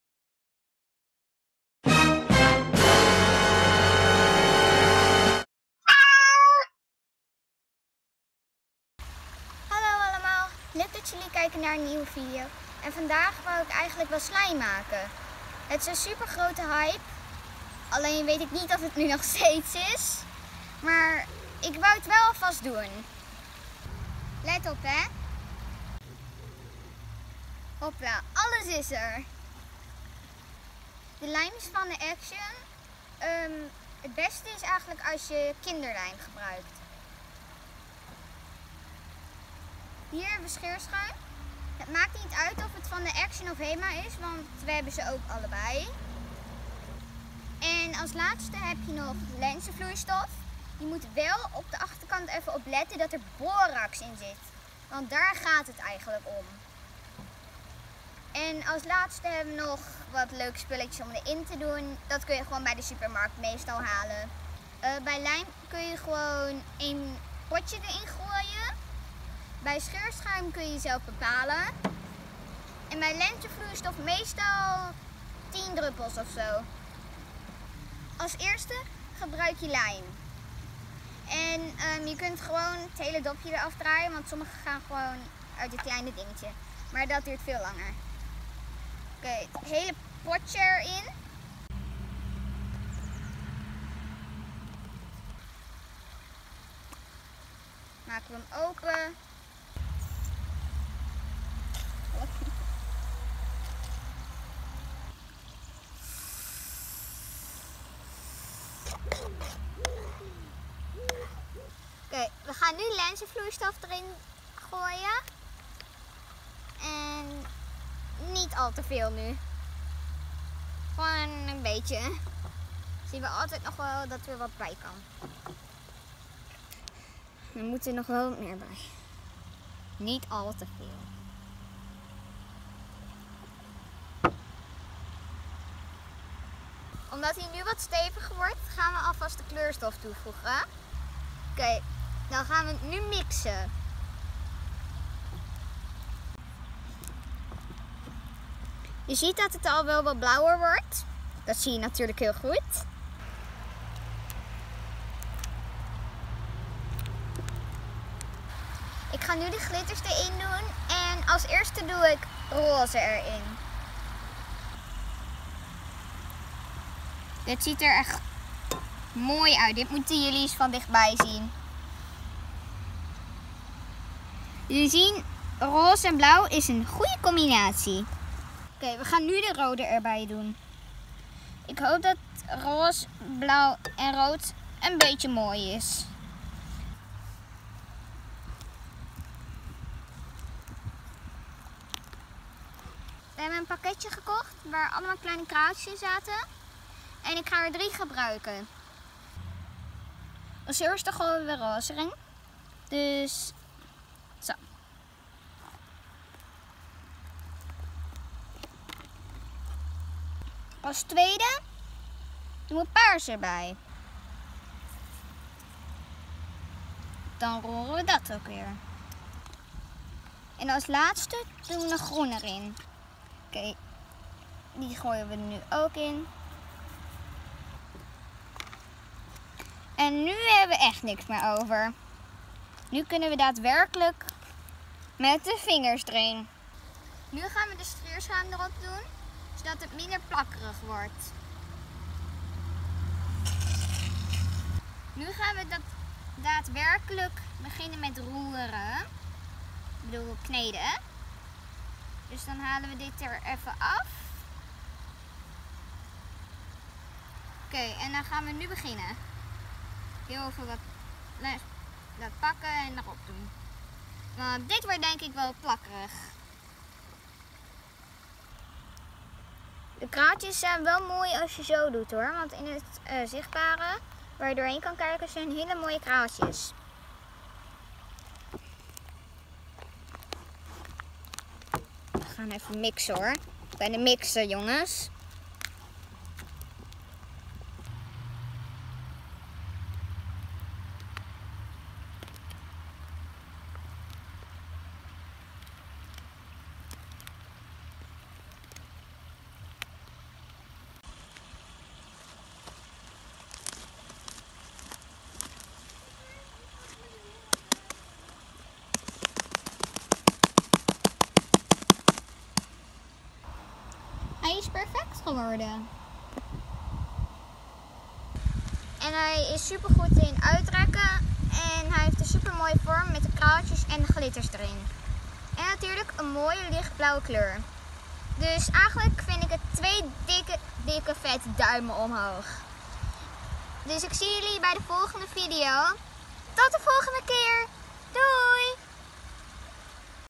Slijm Maken - Vlog #2
Vandaag gaan we slijm maken, dit leek me al een tijdje ontzettend leuk om over te vloggen. Hopelijk vonden jullie het een leuke video. Laat je me weten of jullie slijm ook zo goed gelukt is? ♥ Famke

PS. Om zelf slijm te maken heb je nodig:
1. Kinderlijm (Action)
2. Scheerschuim (Action of Hema)
3. Lenzenvloeistof met Borax! (Action)
4. Kleurstof en eventueel glitters en kraaltjes